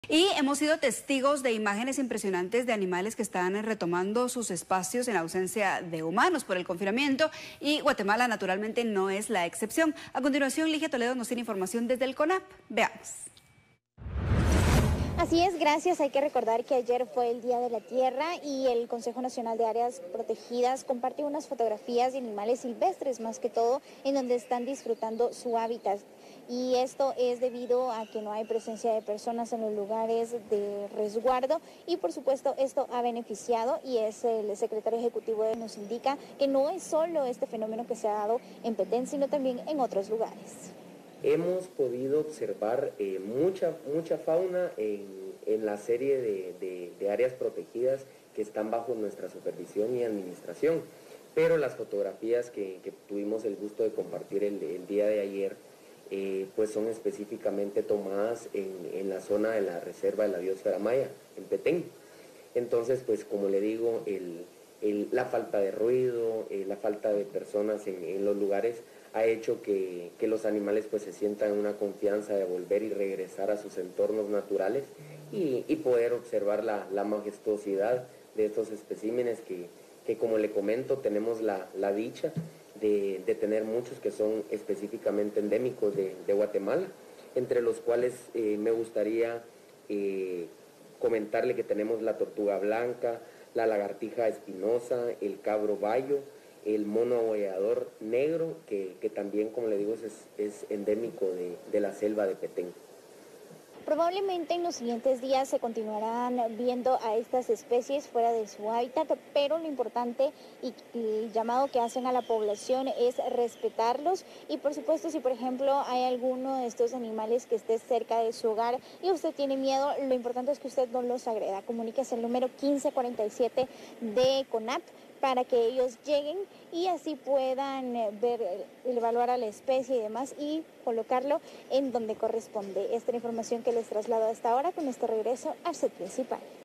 Animales silvestres aparecen en Petén
Durante las últimas horas, el Consejo Nacional de Áreas Protegidas compartió diversas fotografías de animales silvestres en la zona de biosfera maya del Municipio de Petén. CONAP publicó fotografías de bellas especies